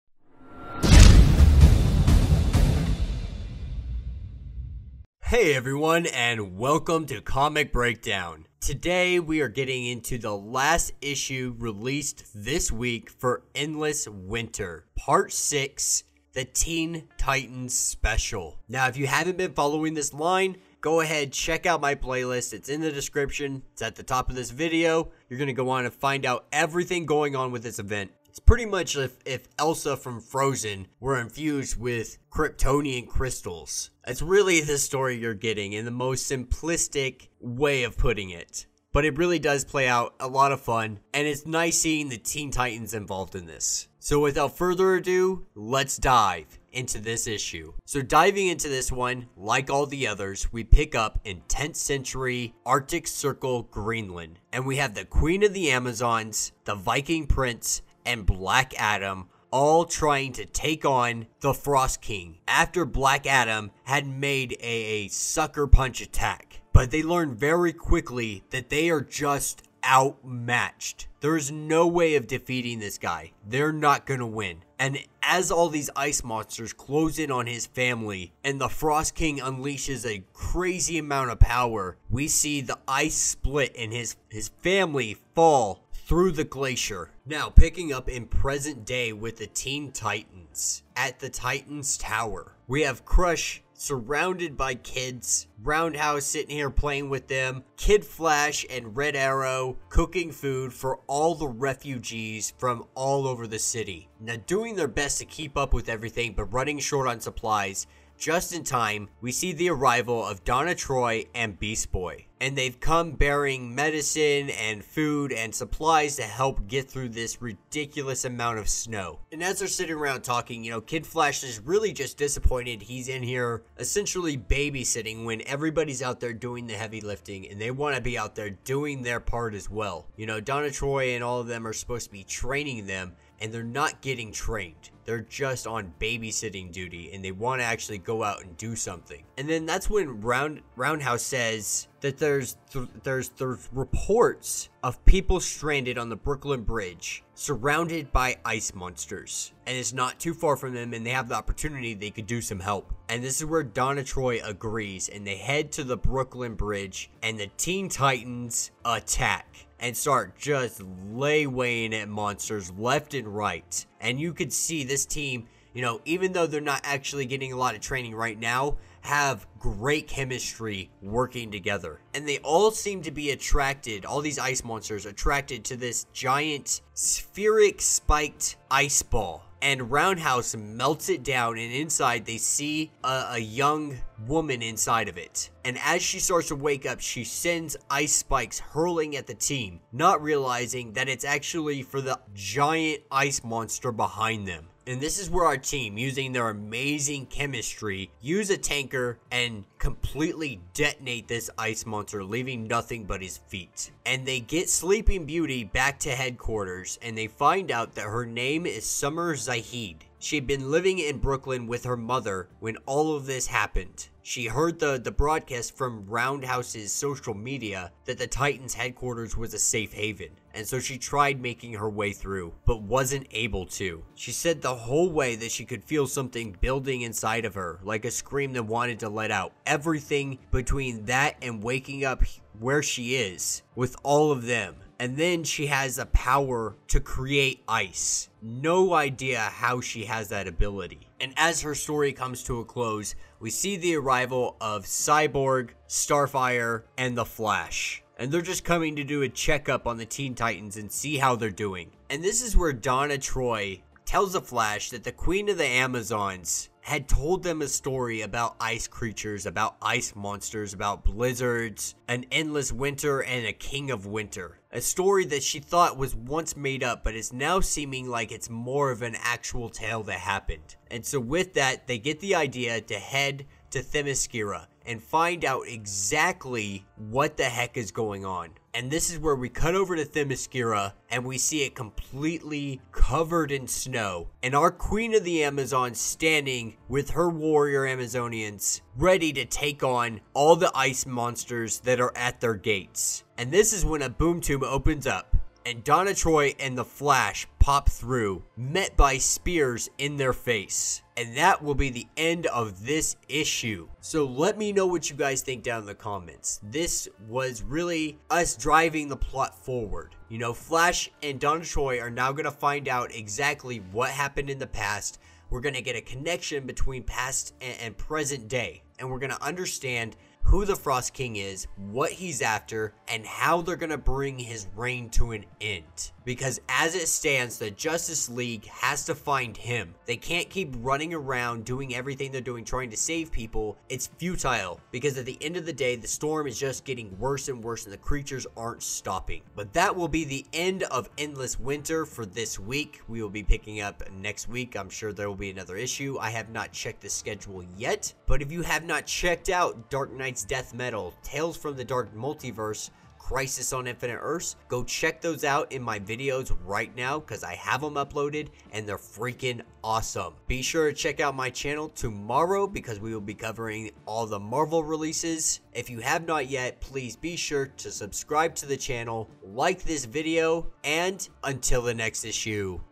Endless Winter: Teen Titans (part 6, Special issue, 2020)
In this video we will be going over, Endless Winter: Teen Titans, Special issue (2020), published by D.C. Comics! All rights are owned by the respective companies.

“Endless Winter” chapter six! Crush, Roundhouse, Kid Flash, and Red Arrow embark on a bonding exercise with new partners from the original New Teen Titans: Donna Troy and Beast Boy? Everyone’s limits will be tested as they brave the challenges of holiday shopping in Manhattan! And then, the city is buried in snow! When they discover a teen whose powers are triggered by the extreme cold, the team must decide if this new arrival is a future hero or villain. And in the past, Black Adam battles the Frost King with devastating consequences

thanks for checking out Comic Breakdown! Giving you the latest in comic book reviews and commentary.

    For the full story, buy the comic and support the industry.